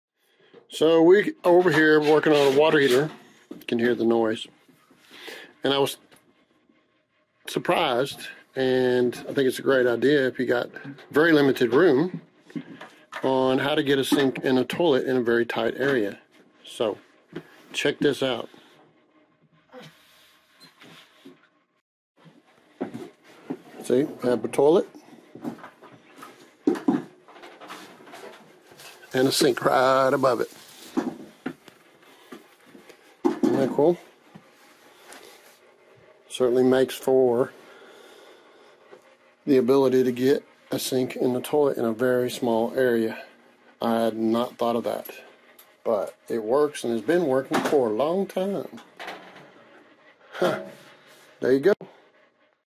Sink And Toilet In Small Bathroom
New way to save space for a bathroom. Something Phil had never thought of trying.

Transcript:
So we are over here working on a water heater. You can hear the noise. And I was surprised and I think it's a great idea if you got very limited room on how to get a sink in a toilet in a very tight area. So check this out.

See you have the toilet and a sink right above it. Isn’t that cool? Certainly makes for the ability to get a sink in the toilet in a very small area. I had not thought of that, but it works and has been working for a long time. Huh, there you go.